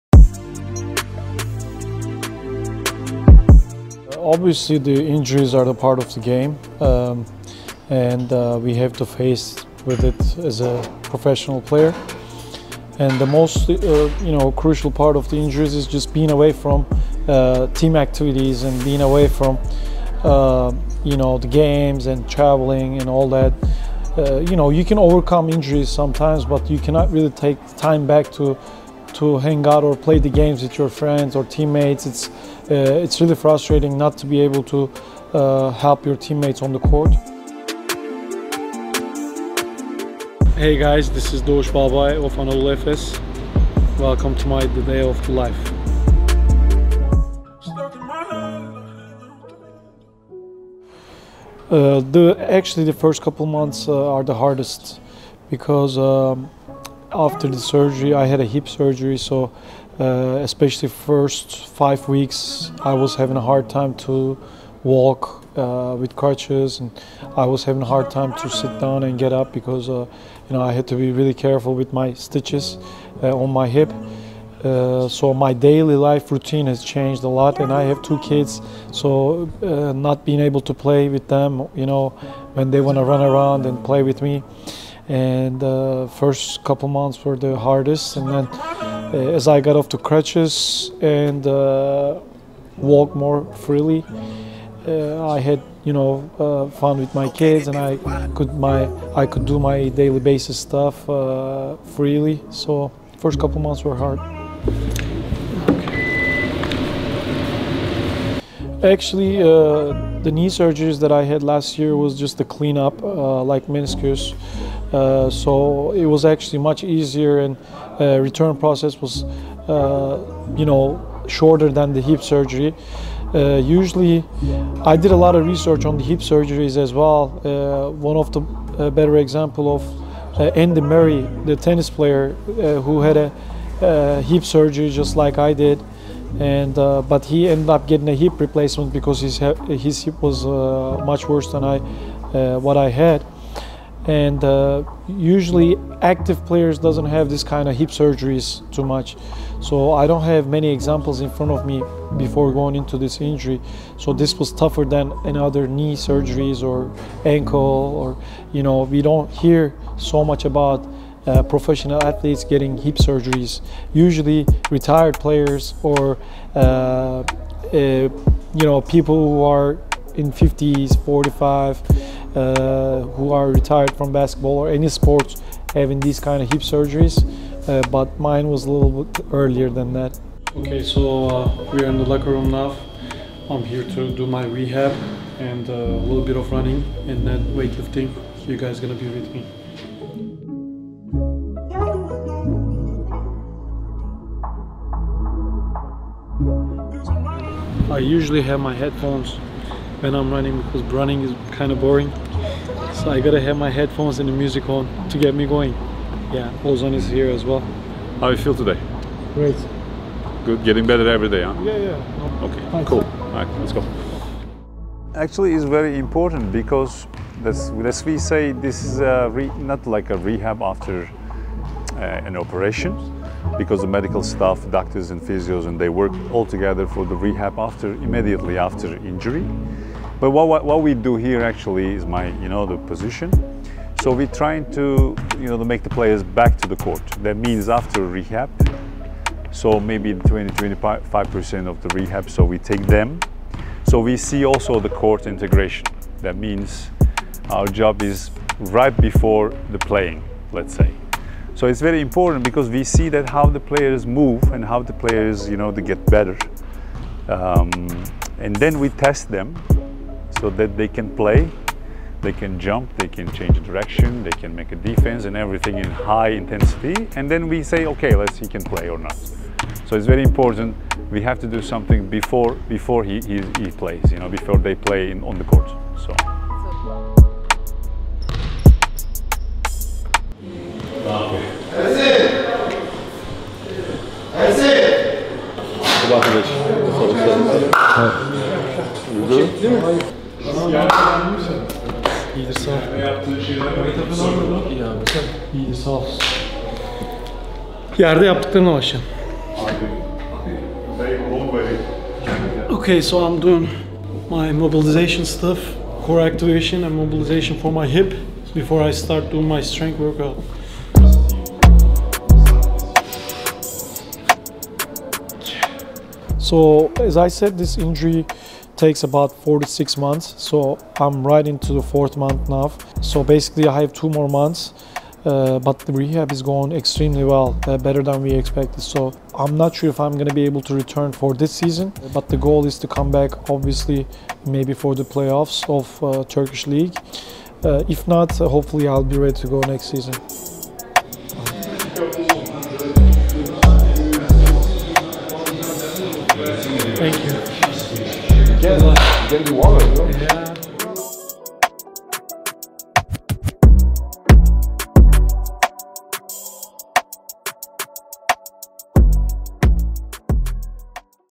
ELPA series: "A day in life" by Dogus Balbay
While the big lights are on the players who will be at the Final 4 in Kaunas, injured players on other teams don't rest.

This is "A day in life" of Dogus Balbay, who is rehabbing from his long-term injury. Follow him through ELPA’s camera.